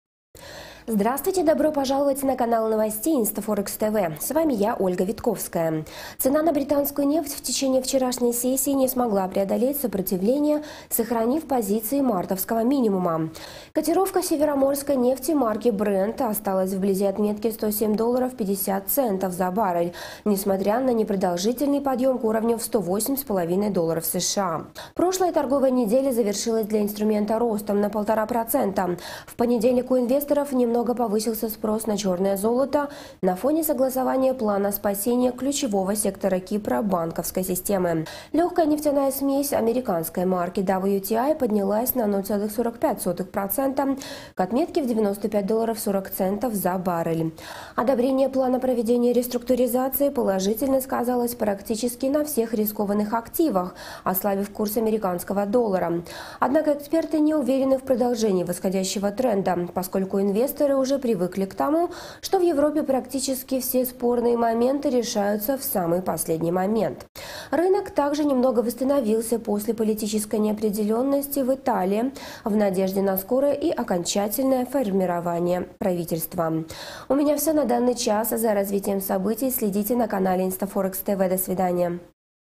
Новости InstaForex 26 Марта. Реакция товарного рынка нефти
Новости InstaForex 26 Марта. Реакция товарного рынка нефти на согласование реструктуризации кипрских банков